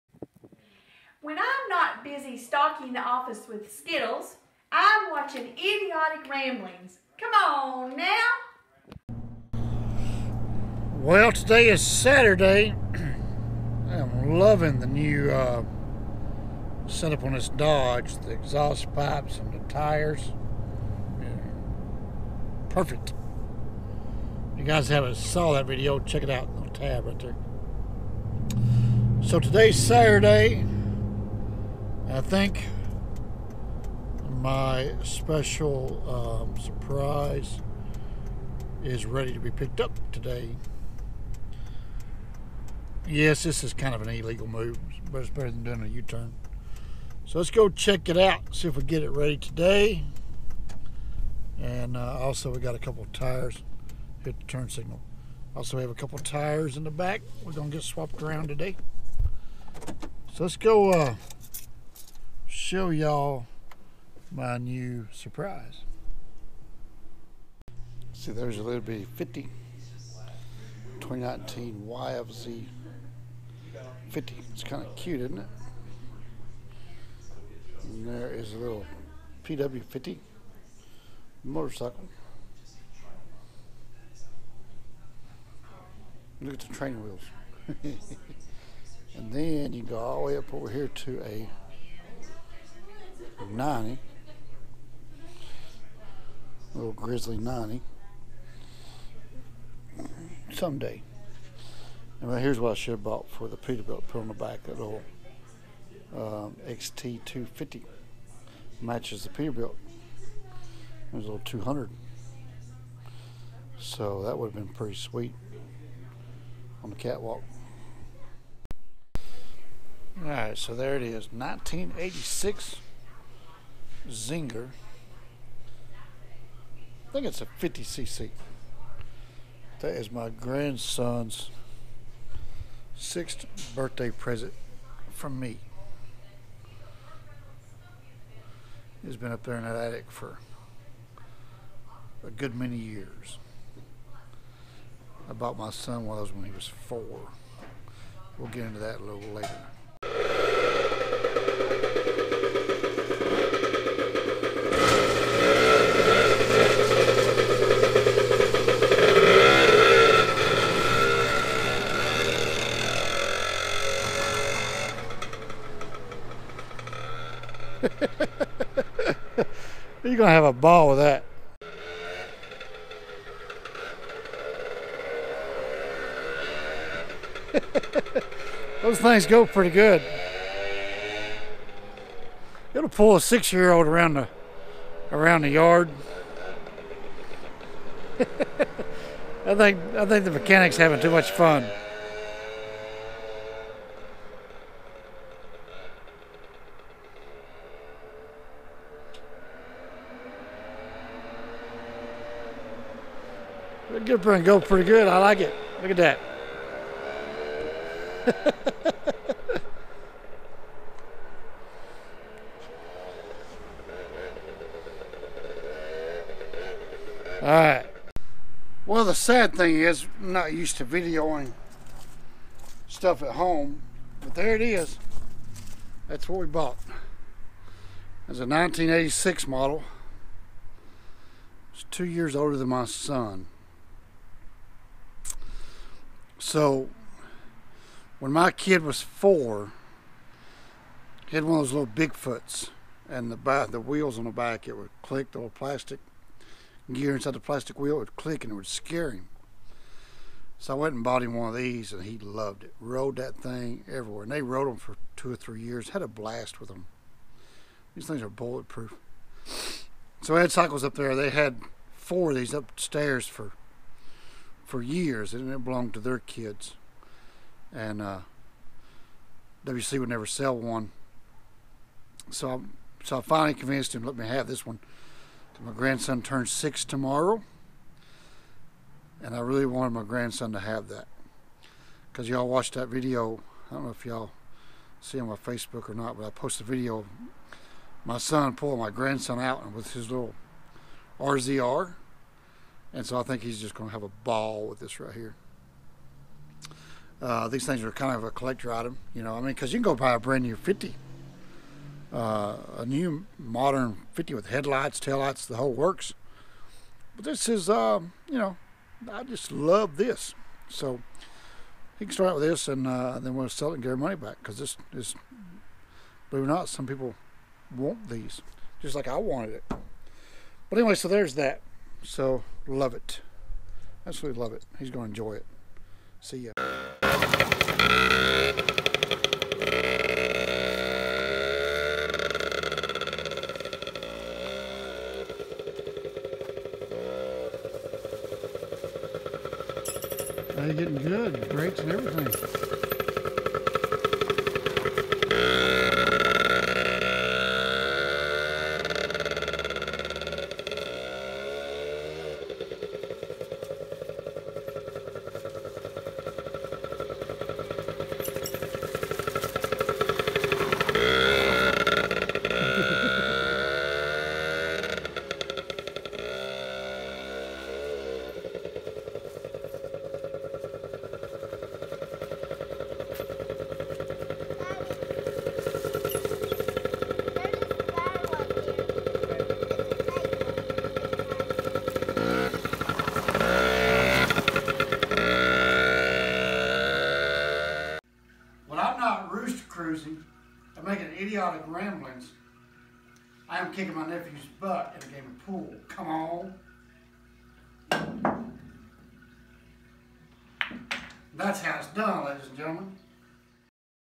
Scored A 33 Year Old Yamaha Zinger 50 cc
I have spent years looking for one of these and I always knew where a couple was but they just wouldn't sell me one until I mentioned it was for my Grandsons Birthday.

Dollar Shave Club Link